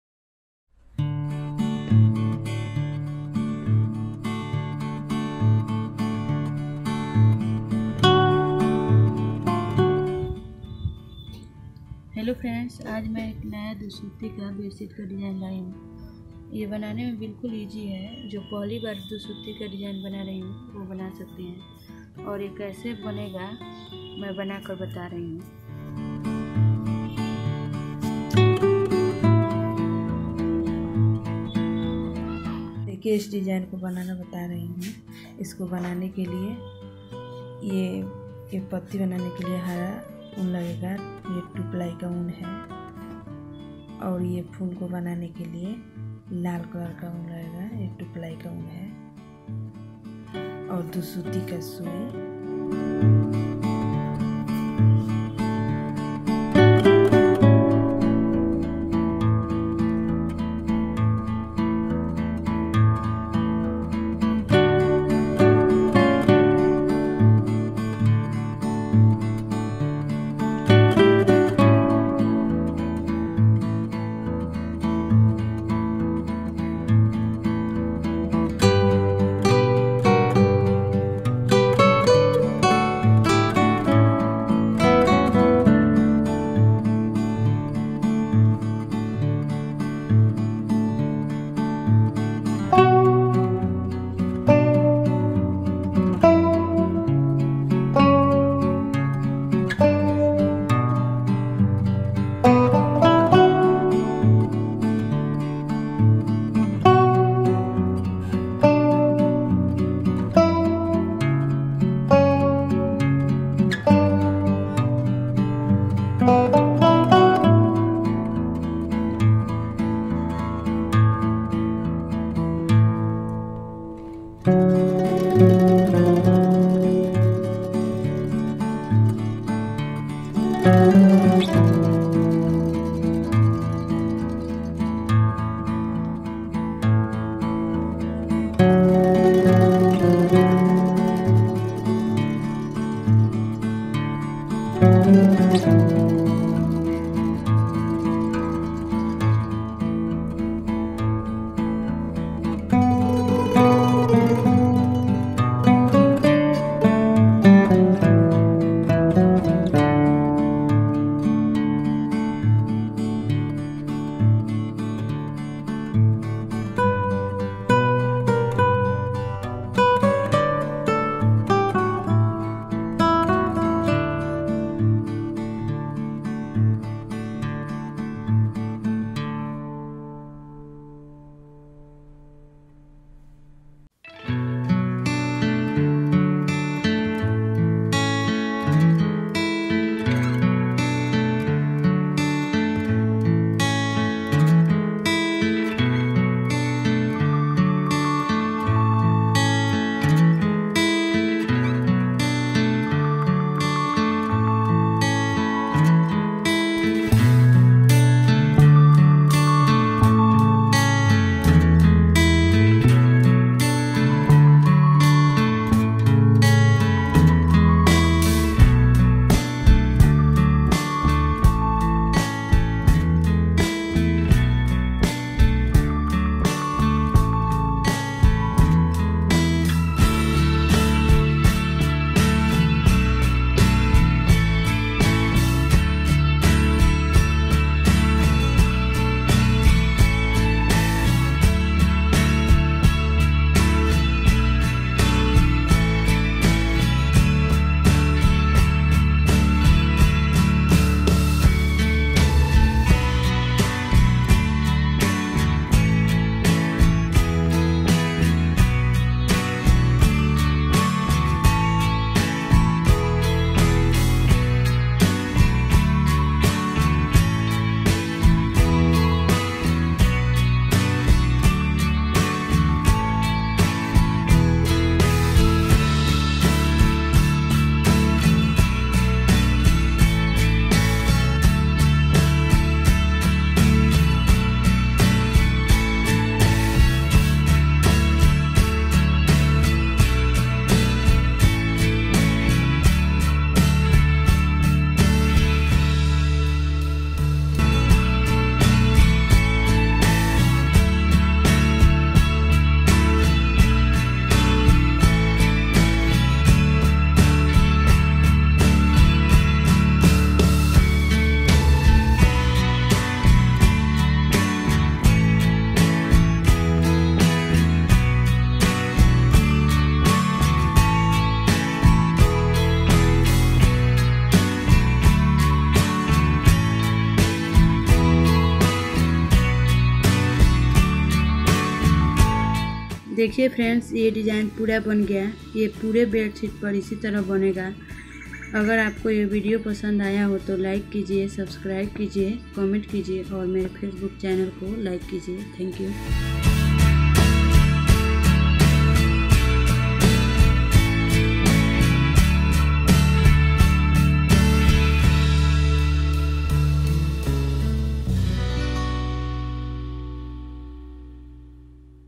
New Dusuti Bedsheet Design In Hindi By Desi Design.
Is video me mai ek our new dusuti design bnake btayi hu jo ki bhut hi aasan hai to please is video ko dekhe our dusuti design bnana sikhe.